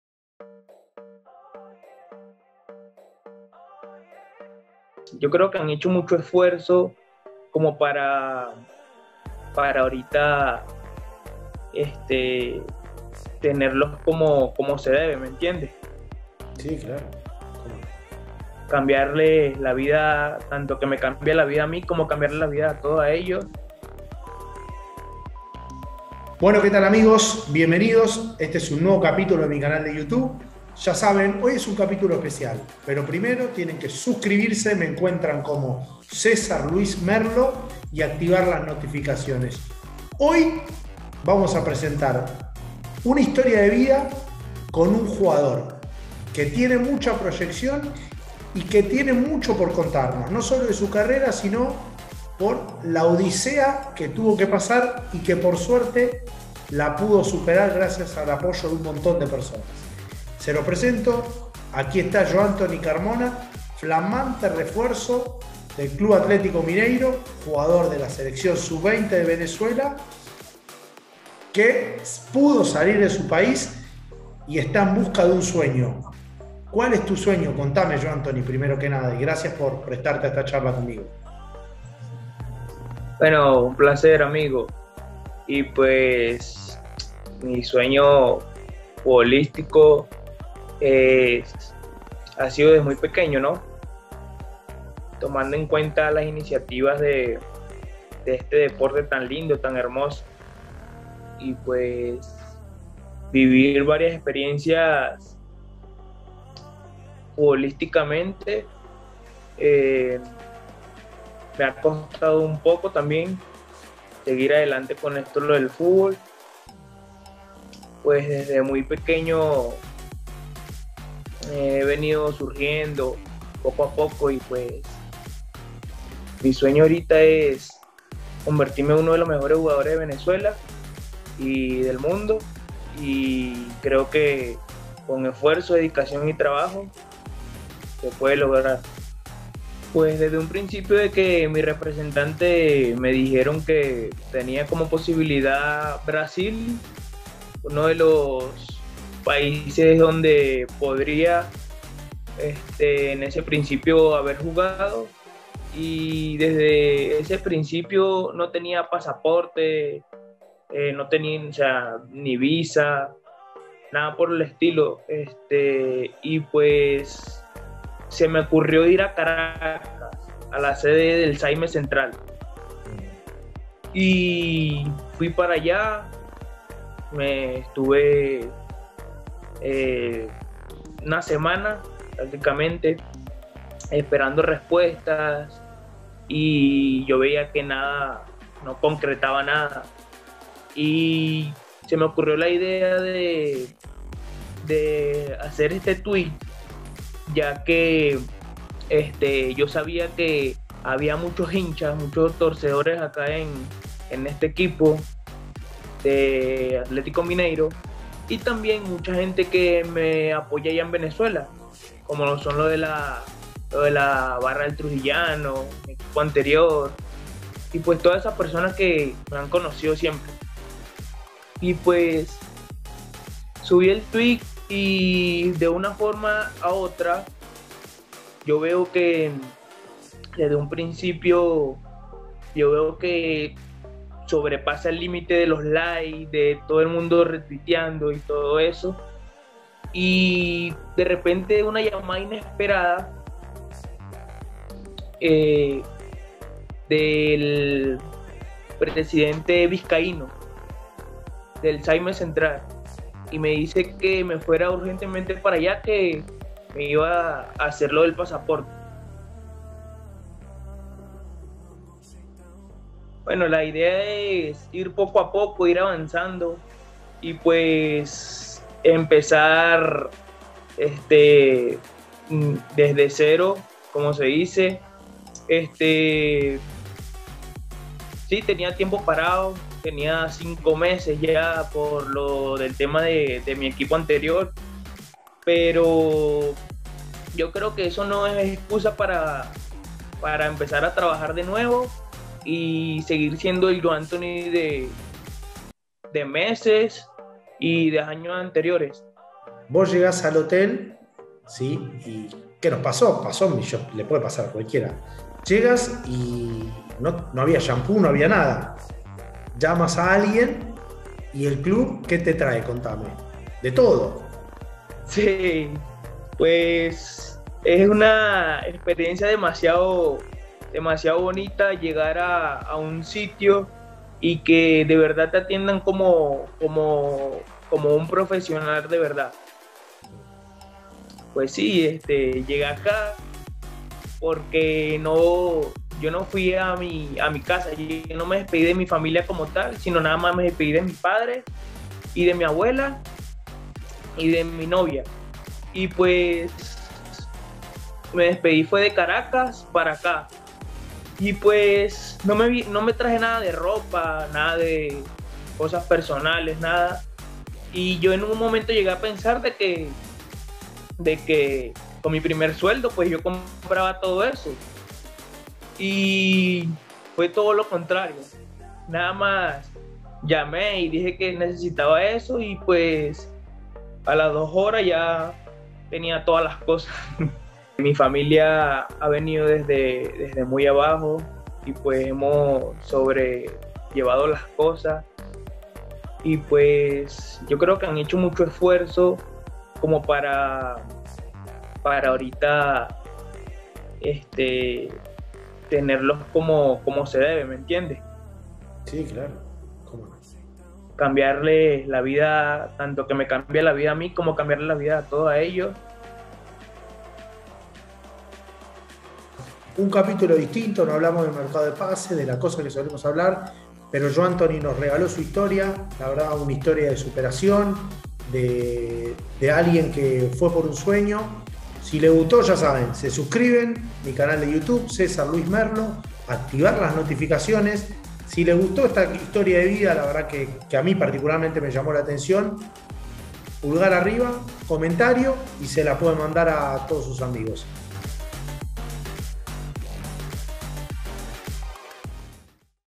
JOANTONY CARMONA, REFUERZO DEL MINEIRO: PASAPORTE A LA ILUSIÓN
JoantonyCarmona necesitó de la ayuda de todos los torcedores de AtleticoMineiro y de la comunidad de Twitter para convertirse jugador del equipo brasileño. En una entrevista imperdible junto a César Luis Merlo, el jugador cuenta su historia de vida y todo lo que tuvo que pasar en Venezuela hasta conseguir el pasaporte.

Edición de foto de portada y video a cargo de Federico Ferster